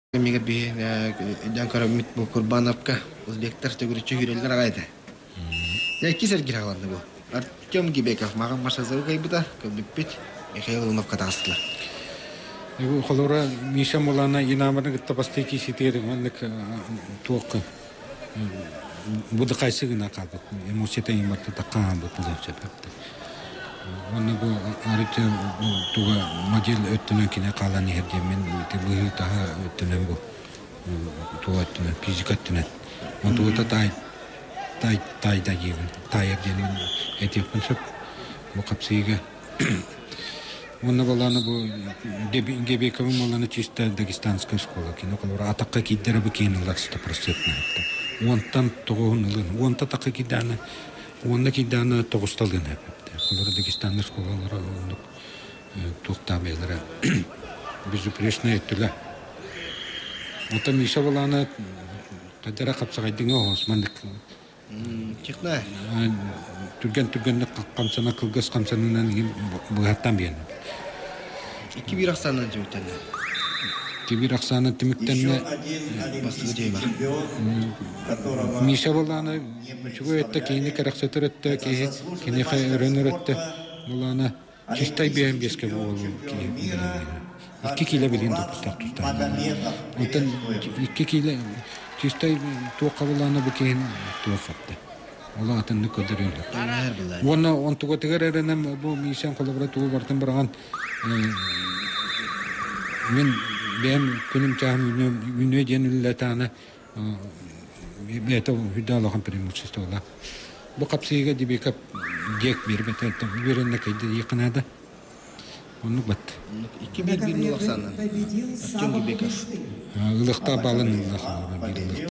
Михаил Иванов (Саха) - Артем Гебеков (Россия)
Международный турнир серии Голден Гран-при «Иван Ярыгин» 2011 г. Красноярск
55 кг. 2 круг